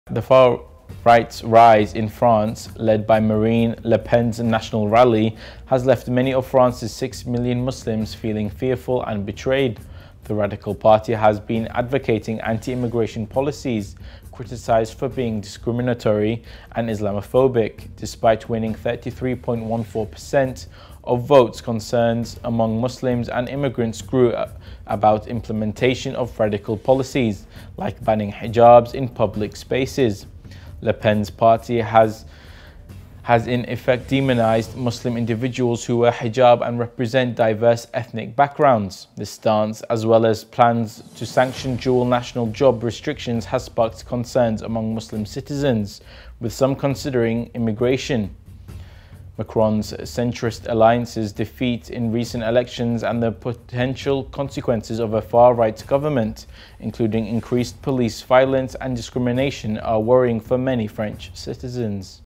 Fears Grow Among French Muslims as Far-Right Eyes Victory in Elections
The far-right's rise in France, led by Marine Le Pen's National Rally, has left many of France's six million Muslims feeling fearful and betrayed.
The radical party has been advocating anti-immigration policies criticized for being discriminatory and Islamophobic.
Despite winning 33.14% of votes, concerns among Muslims and immigrants grew about implementation of radical policies, like banning hijabs in public spaces.
Le Pen's party has in effect demonized Muslim individuals who wear hijab and represent diverse ethnic backgrounds.
This stance as well as plans to sanction dual national job restrictions has sparked concerns among Muslim citizens, with some considering emigration.
Macron's centrist alliance's defeat in recent elections and the potential consequences of a far-right government, including increased police violence and discrimination, are worrying for many French citizens.